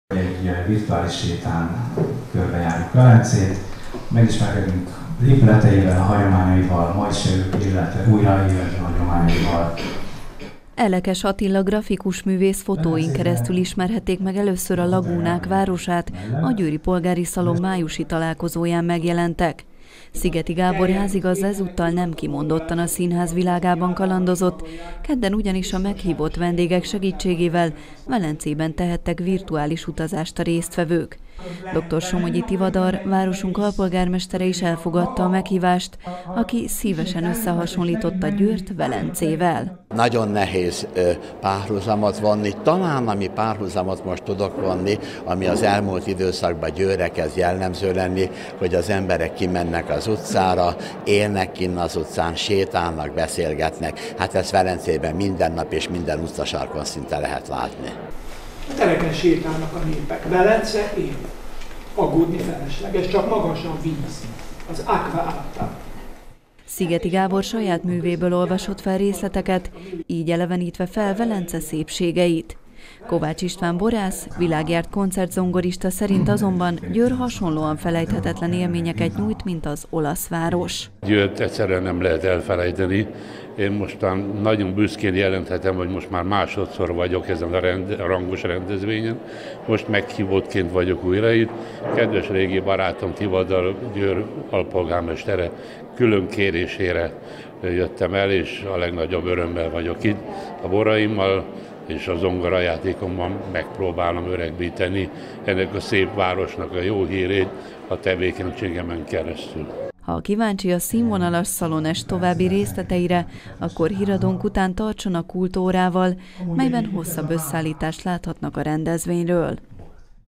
Utazzunk Velencébe
A Győri Polgári Szalon májusi találkozóján is virtuális utazásra invitálta vendégeit Szigethy Gábor. A házigazda ezúttal nem kimondottan a színház világában kalandozott, hanem a lagúnák városában, hiszen ezúttal Velence volt a téma. Az esten meghívott vendégek meséltek „Adria királynőjéről", így dr. Somogyi Tivadar, városunk alpolgármestere, Elekes Attila grafikusművész, de Kovács István borász, világjárt koncertzongorista is hozzájárult ahhoz, hogy színvonalas szalon-est szülessen.